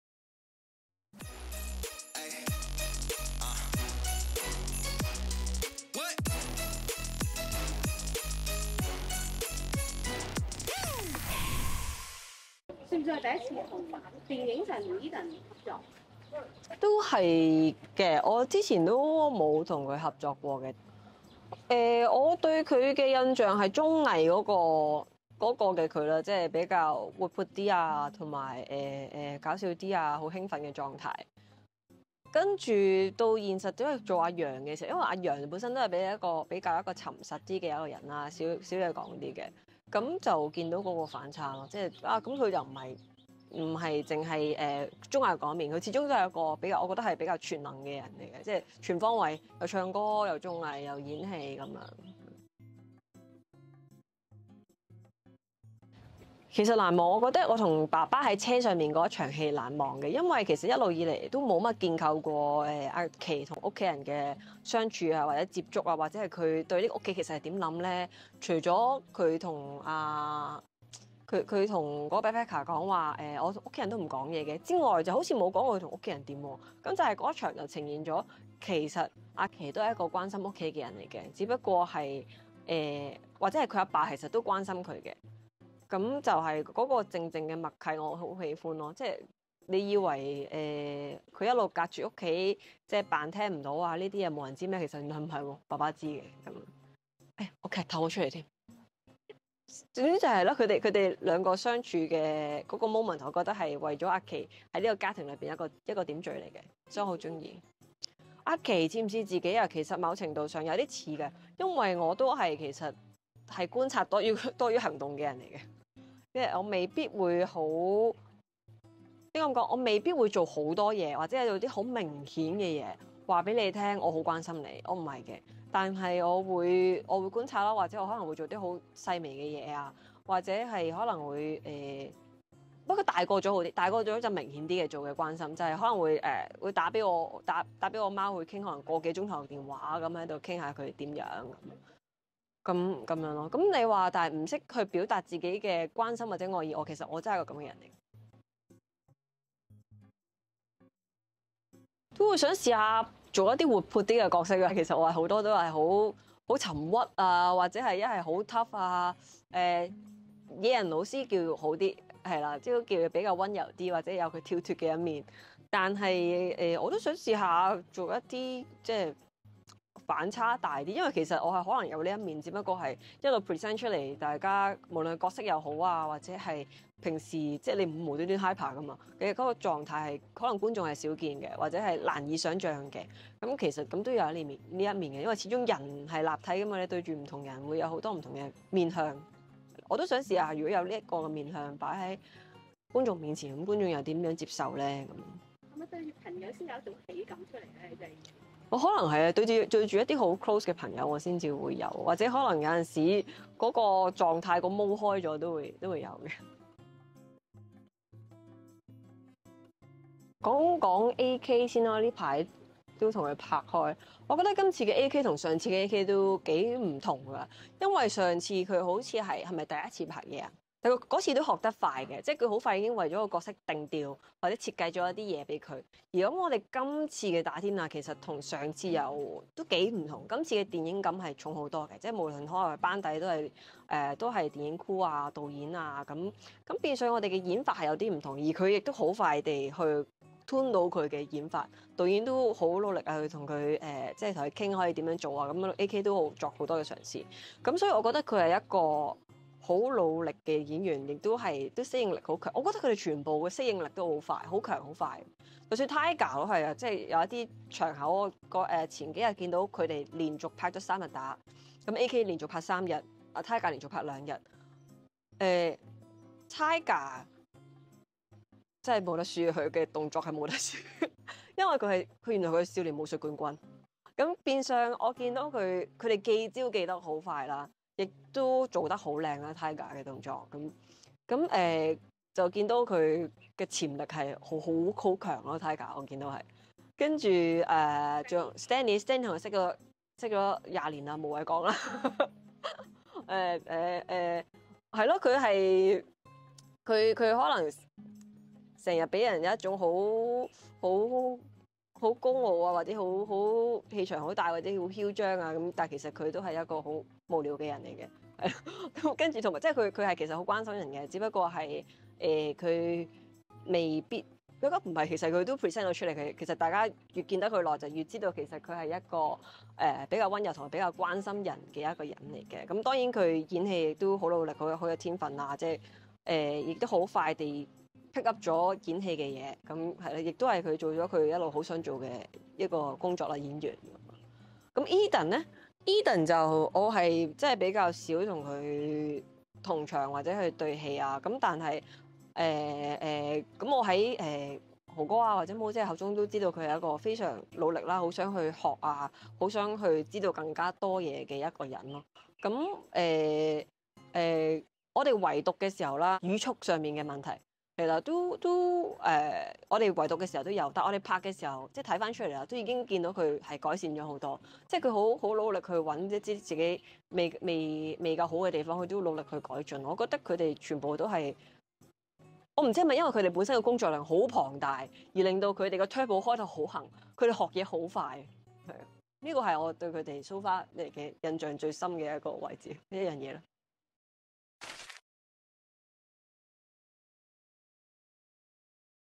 過時．過節丨專訪丨談善言難忘與謝君豪演父女丨談合作過MIRROR成員 讚適應力強丨AK再合作大不同丨Tiger無得輸丨Stanley看似高傲 內心溫柔丨Edan努力改進丨田木集作
📌 支持「田木集作」詳盡內容請瀏覽影片
📌睇片後可以按Super Thanks❤️ 支持「田木集作」
由新導演曾慶宏改編自身故事的電影《過時．過節》上映逾兩週，票房走勢持續向好，呂爵安（Edan）亦落力到不同戲院謝票。而在戲中演阿陽（Edan飾）家姐阿琪的談善言，早前接受《田木集作》訪問，除表示很喜歡在車廂與謝君豪對話的一場戲，還講到他與幾位MIRROR成員先後合作的感受。足本內容，記得睇片喇！